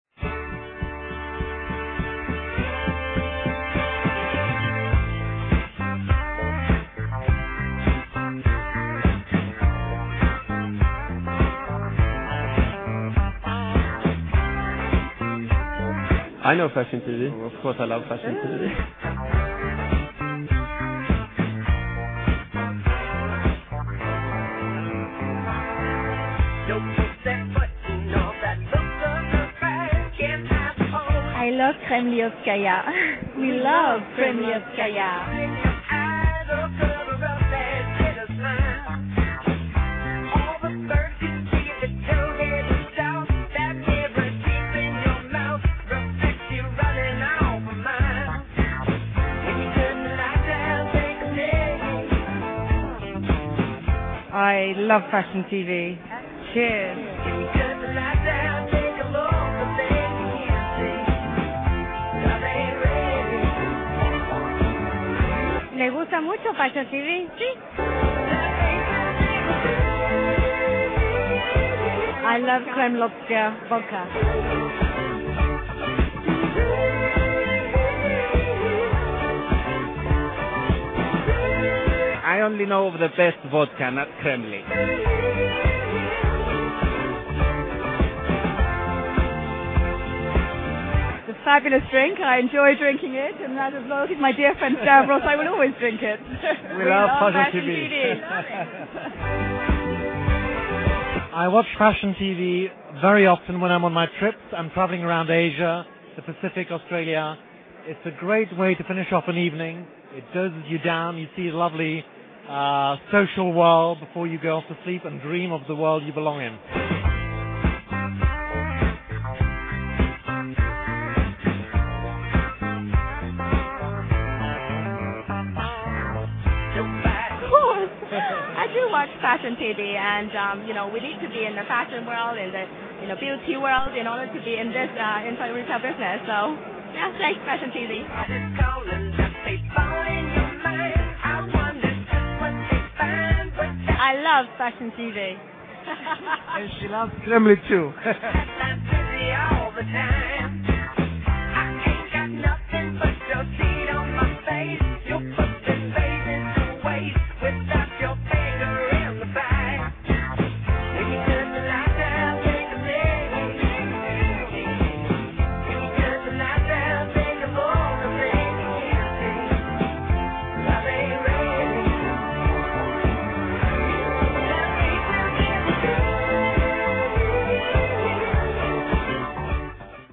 FTV & Kremlyovskaya vodka present Frontier Awards 2006
Fashion TV & Kremlyovskaya vodka present the Frontier Awards 2006, Martinez Hotel, Cannes, France, 24th October 2006